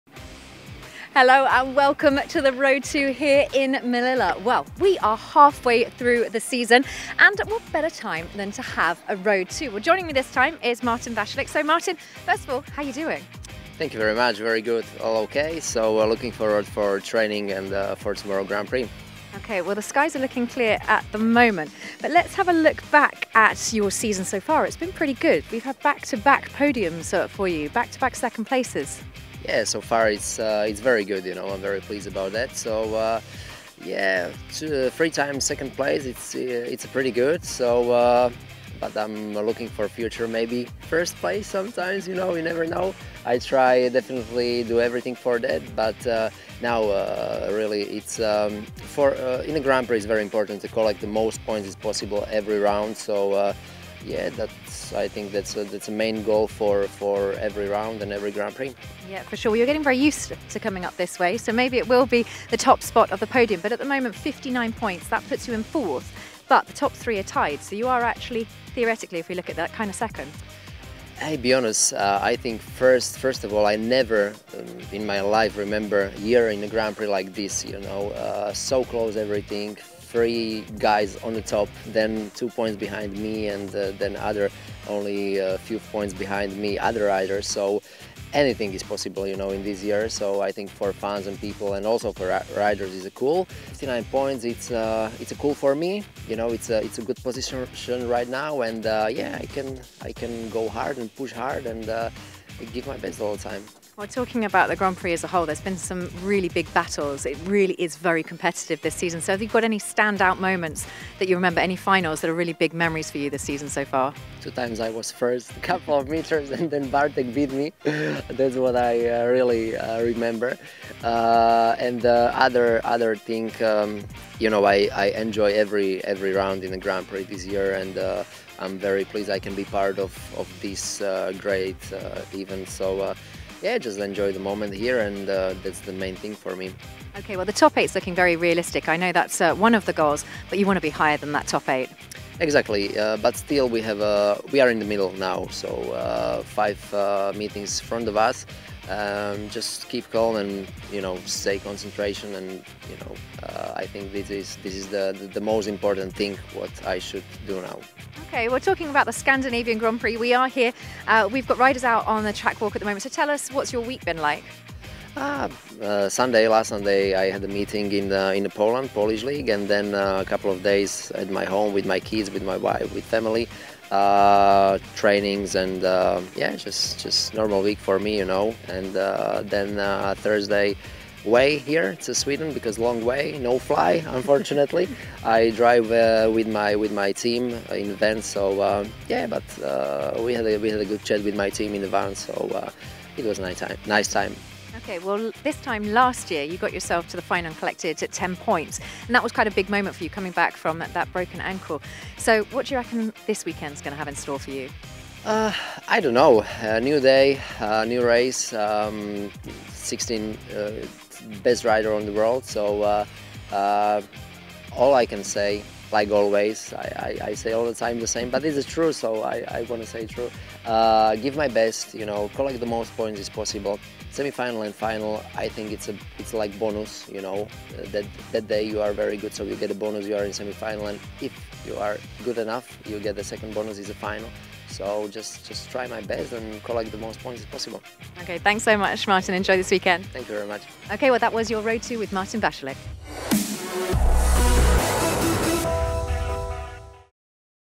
Road to Malilla with Martin Vaculik | Scandinavian SGP
He's enjoying his best FIM Speedway Grand Prix season so far - Kiri Bloore catches up with Martin Vaculik on the Road to Malilla.